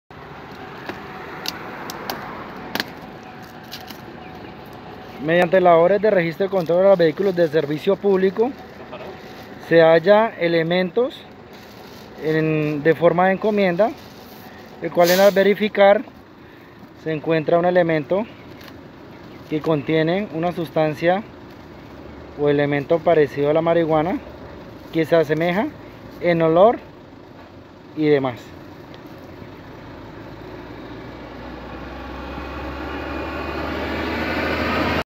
Policía Meta incauta 10 mil gramos de marihuana
Mediante puesto de prevención vial ubicado en el sector de Yucao, uniformados de la seccional de Tránsito y Transportes del Departamento de policía Meta logran la incautación de 10 mil gramos de marihuana tipo Creepe.

El hecho se registró en horas de la noche momentos en que los efectivos de transito detienen un vehículo de pasajeros tipo microbús, al solicitar un registro al automotor, mediante la modalidad de encomienda se logra ubicar el alcaloide avaluado en más de 6 millones de pesos, el cual fue dejado a disposición de  la fiscalía local de Puerto Gaitán meta por el delito de Fabricación, Tráfico y Porte de Estupefacientes.